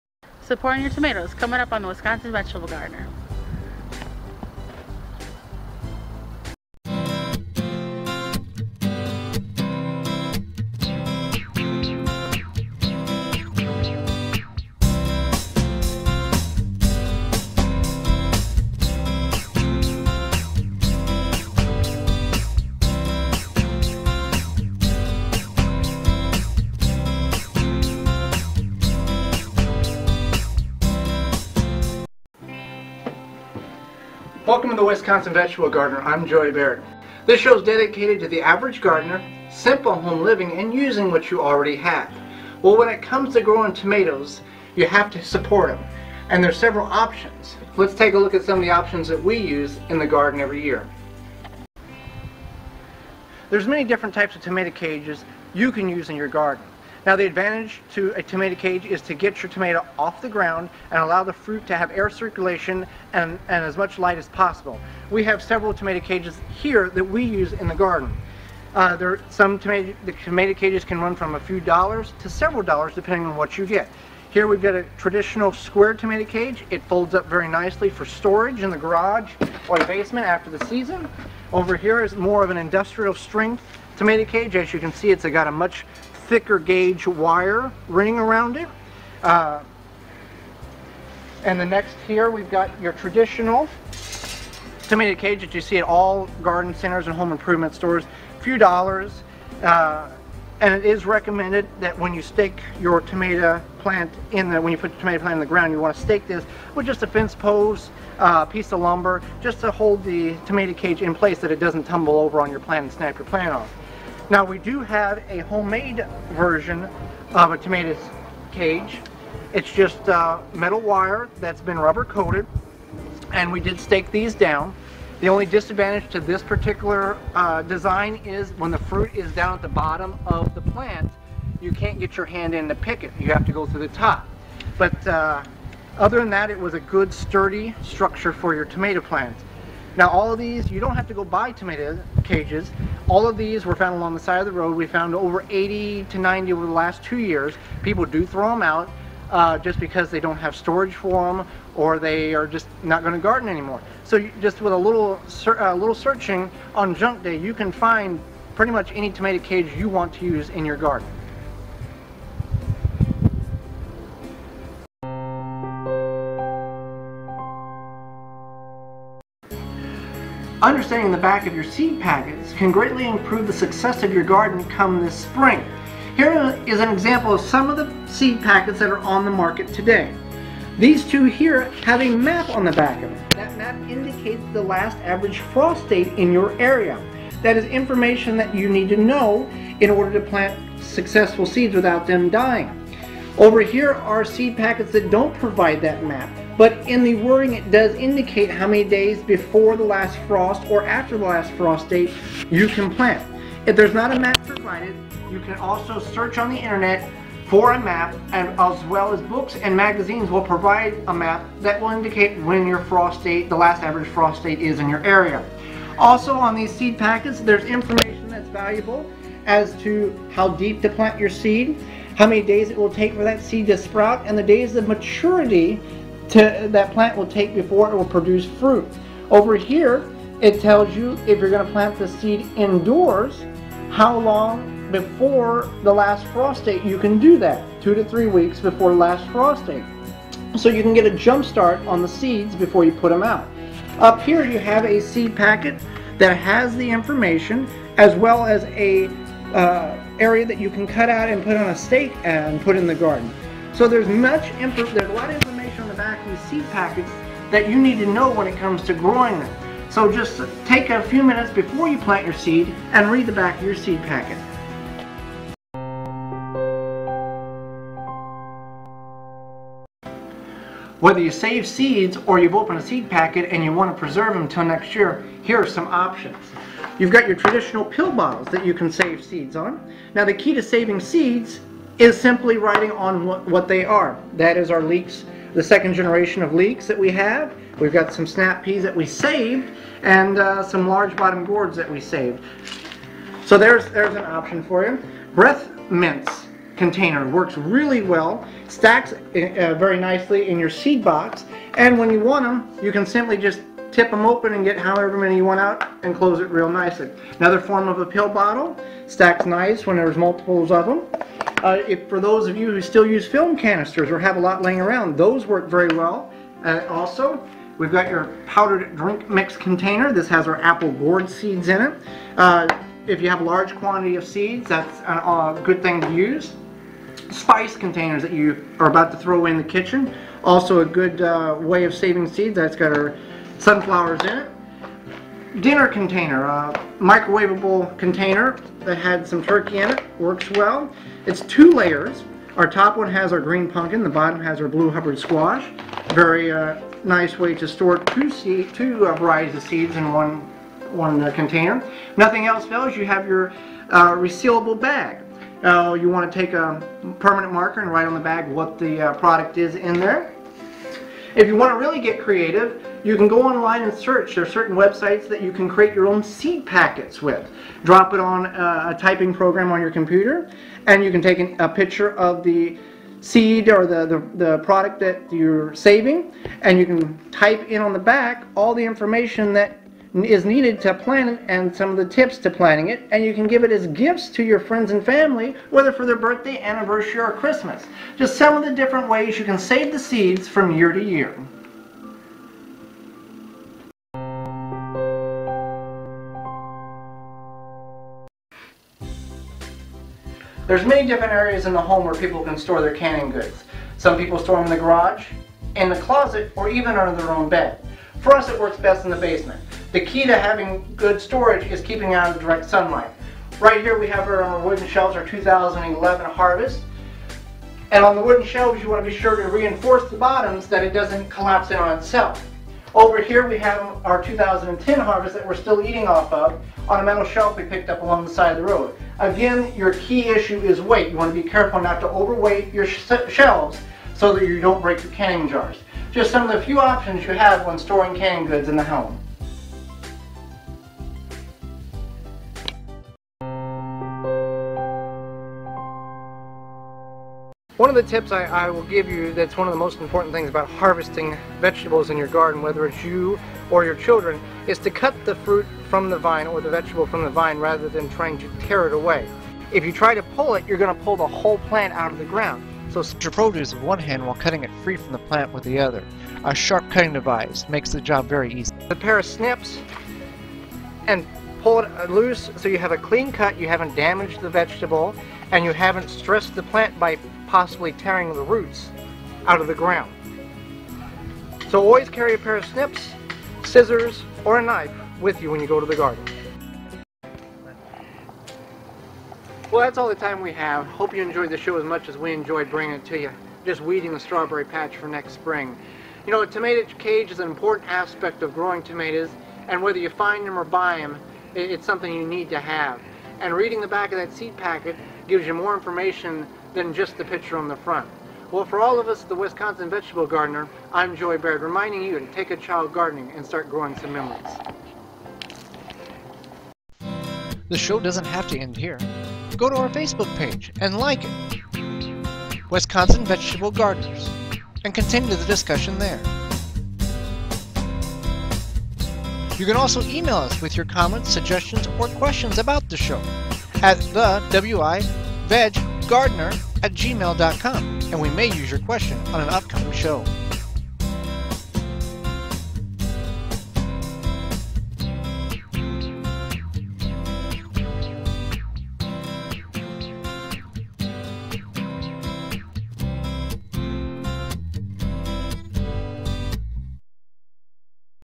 The Wisconsin Vegetable Gardener Show 6
Tomato cages, reading the back of your seed packets, storing canning goods different ways to store seeds